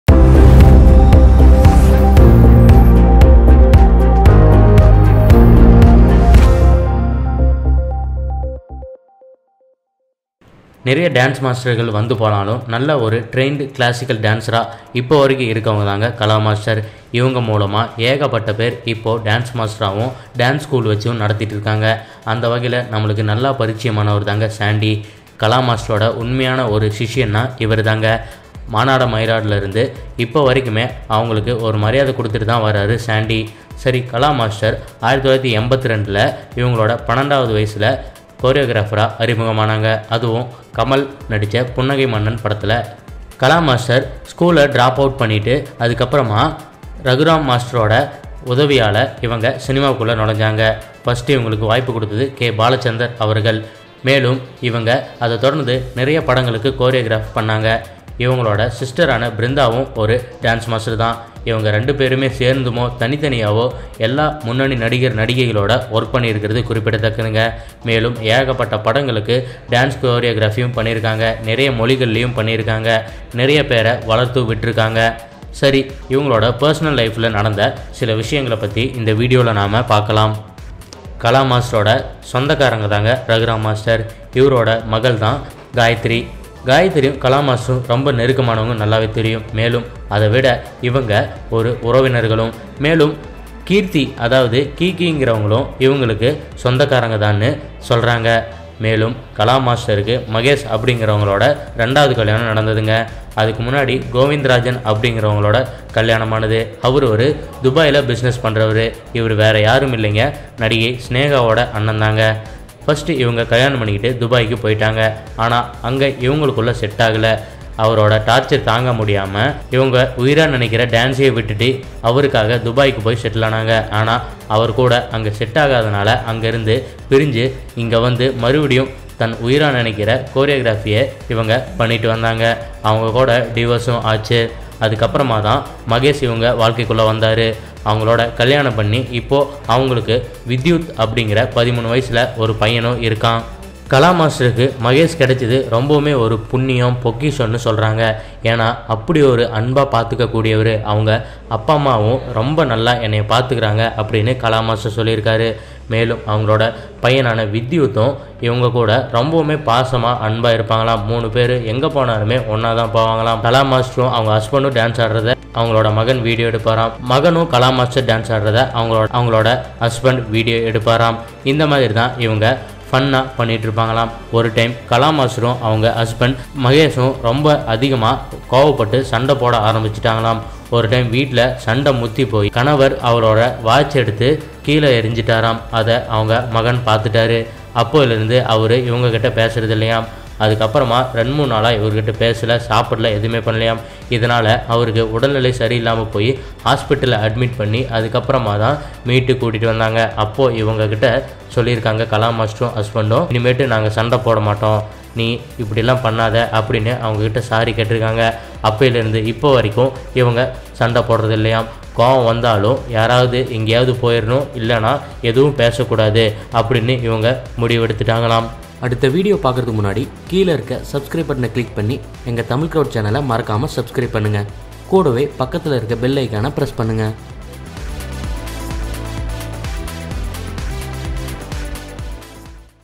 கலா மாஸ்டர் வீட்டில் மகனுக்கு ஏற்பட்ட விபரீதம்! | Kala master | Husband | Son | Sandy | Family
Sad incident that happened to Kala master!

கலா மாஸ்டர் வீட்டில் மகனுக்கு ஏற்பட்ட விபரீதம்!

Kala is an Indian dance choreographer who has worked in all the regional film industries. She is the director of the Indian reality dance talent show, Maanada Mayilada where she is one of the three judges. Here we are going to see about sad incident that happened to her son!